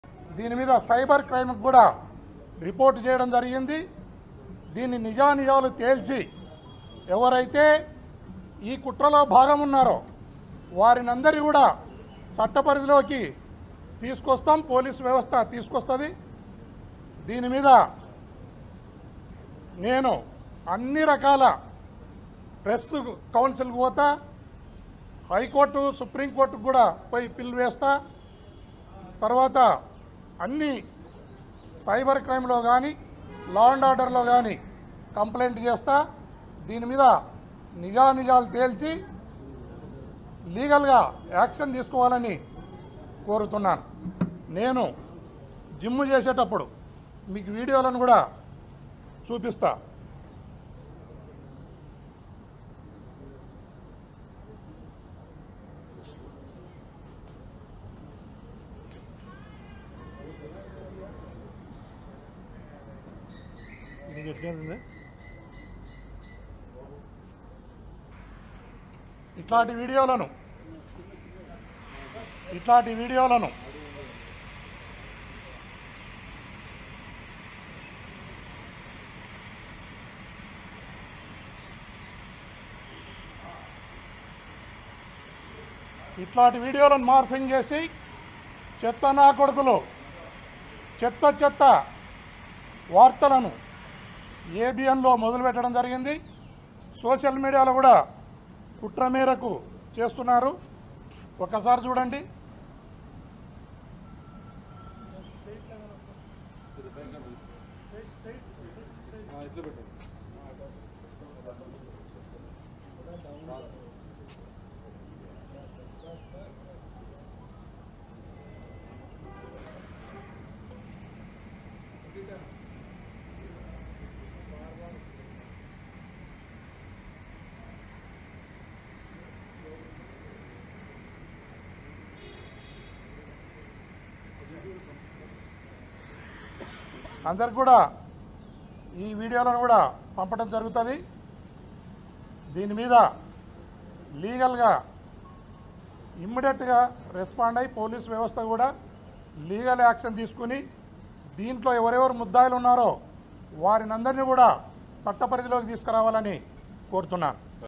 న్యూడ్ వీడియో లీక్ పై స్పందించిన గోరంట్ల మాధవ్ | Gorantla Madhav Latest Press Meet | Around Telugu
Watch Here : న్యూడ్ వీడియో లీక్ పై స్పందించిన గోరంట్ల మాధవ్ | Gorantla Madhav Latest Press Meet | Around Telugu